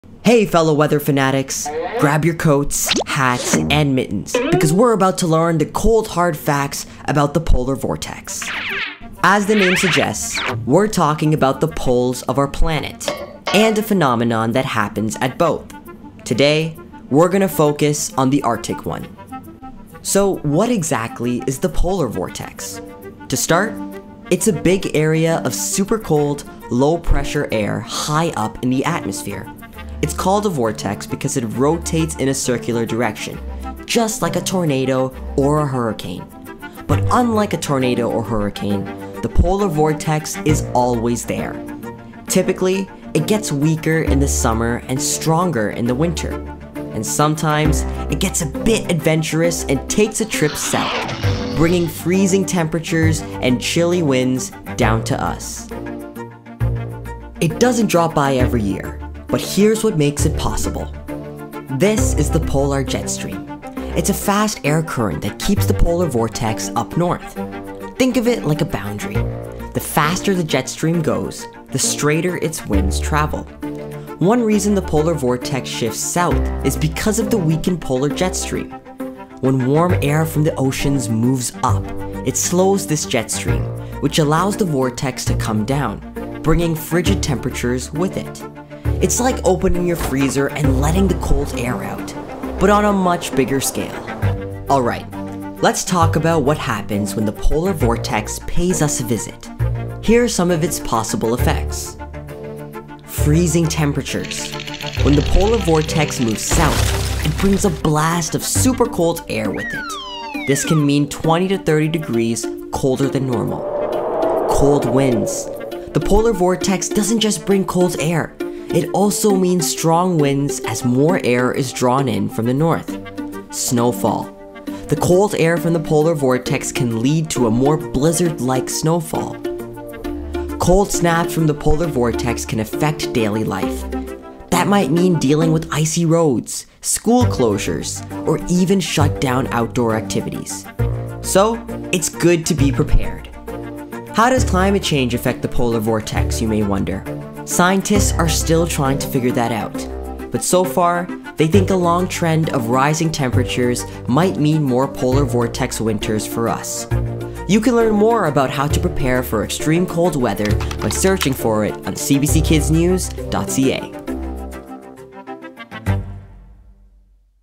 How the Arctic polar vortex can affect winter weather | CBC Kids News
Check out this cool video all about the polar vortex! Learn what it is, how it forms and why it's important. Join us as we explore the science behind this icy phenomenon and discover how it affects weather.

CBC Kids News is a website for kids, covering the information you want to know. Real Kids. Real News.